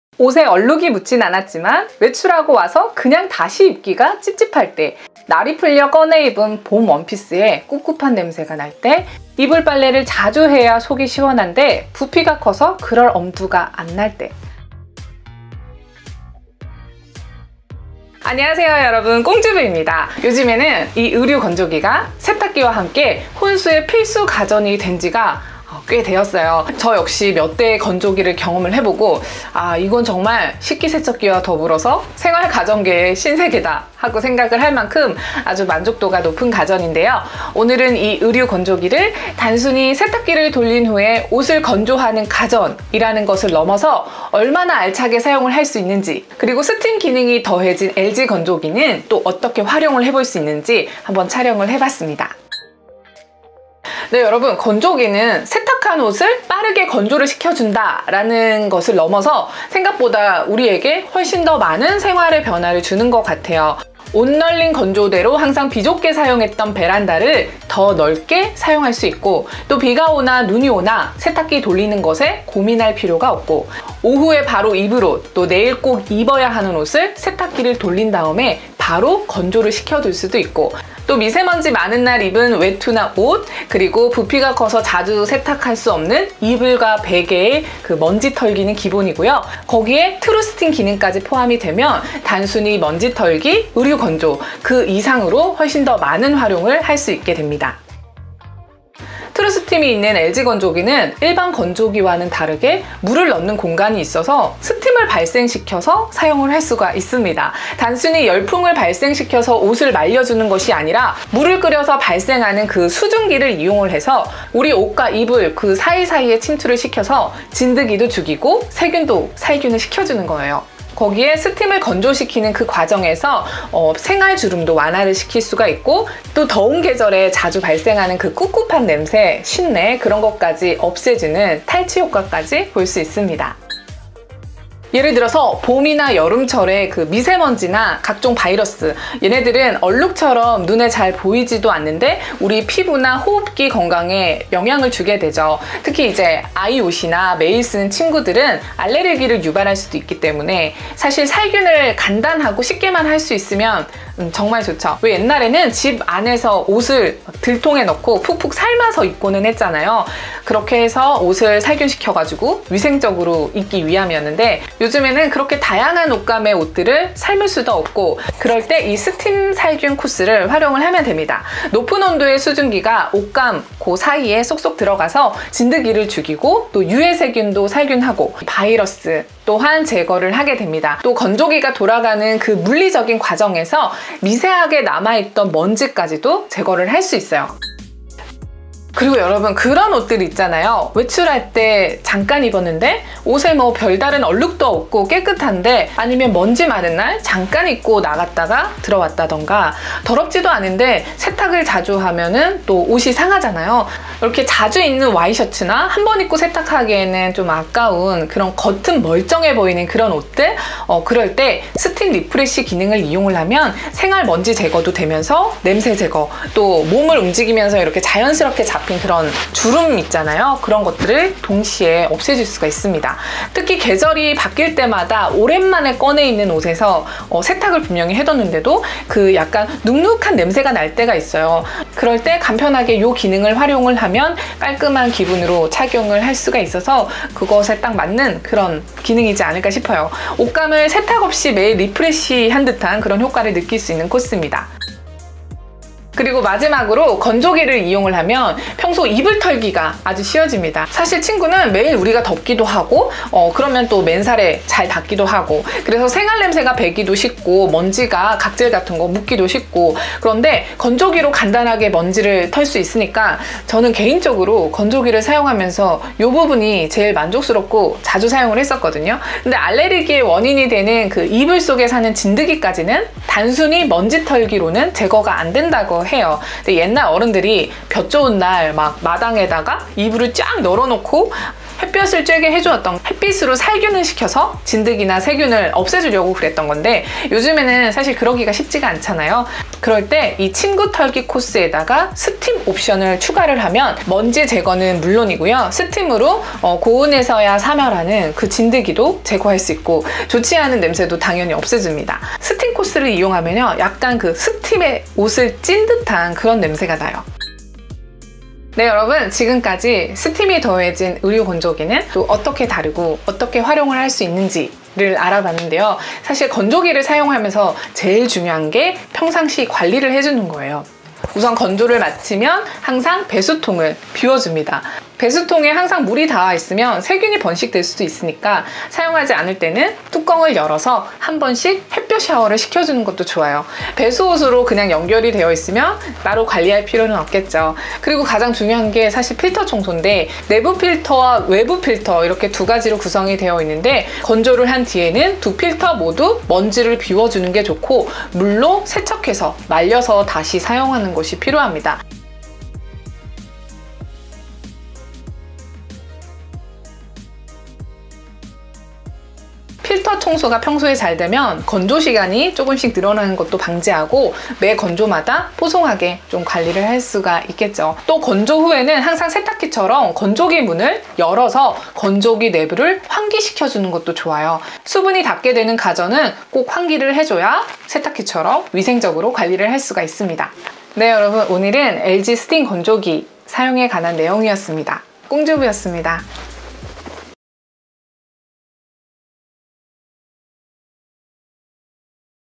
건조기에 어디까지 사용해봤니?! 스팀건조기가 가져온 의외의 찐변화 3가지
여러분:) 이번엔 스팀 건조기에요!
돈 안아까운 가전 Best3에 언제나 빠지지 않는 의류 건조기에 + 스팀기능까지 더해지면 어떻게 활용할 수 있는지 촬영해봤어요.

식기세척기에 이어서 건조기에도 트루스팀이 나오게 한 LG 건조기_
즐겁게 시청하시고 오늘도 좋은 하루 보내시기를. ♡

🥕 이 영상은 LG전자측으로부터 제품과 제작비를 지원받아 제작되었습니다.
🥕 영상이도움 되셨다면면 '좋아요'🙏
🥕 더 많은 영상을 보시고 싶으시면 '구독을' 🤗